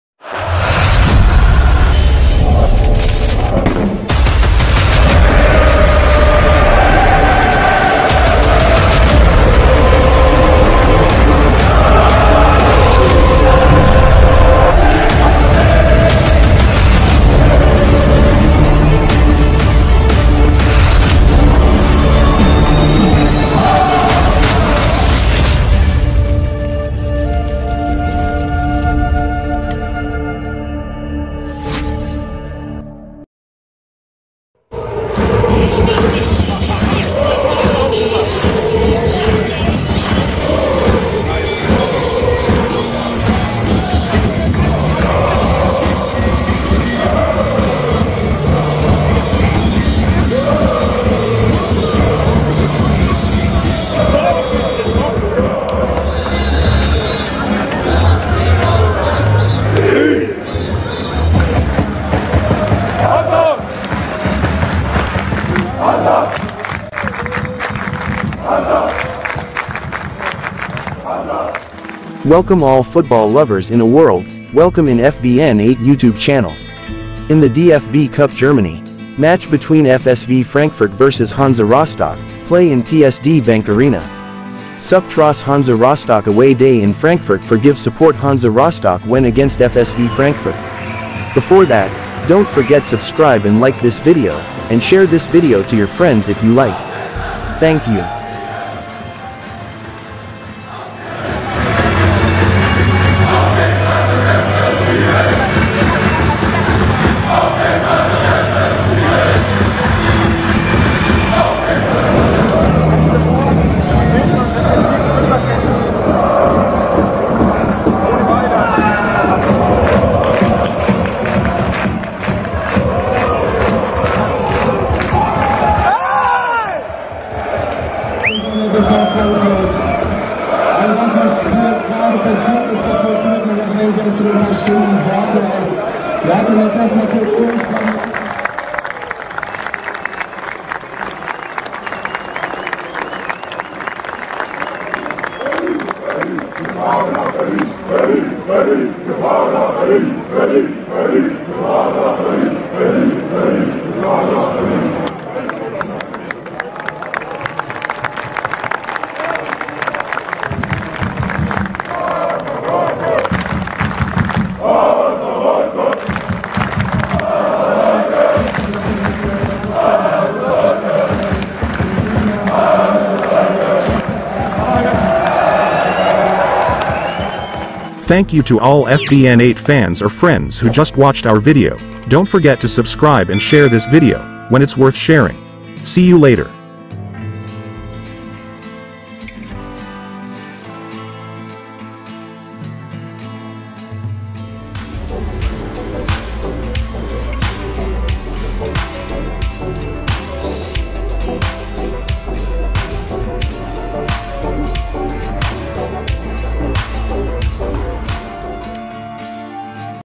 🔴⚪🔵 HANSA ROSTOCK FANS AWAY DAY IN PSD BANK ARENA | FSV Frankfurt vs Hansa Rostock | DFB Pokal
▶️ : FBN8 Channel Youtube.

We will immediately respond to remove this video or we will find another solution together.

Match : FSV Frankfurt vs Hansa Rostock
League : DFB Cup Germany🇩🇪
Score : 1-1 AET (PK 0-3) (14/08/2023)
Stadium : PSD Bank Arena.

About Video :Ultras Fans Hansa Rostock in Frankfurt.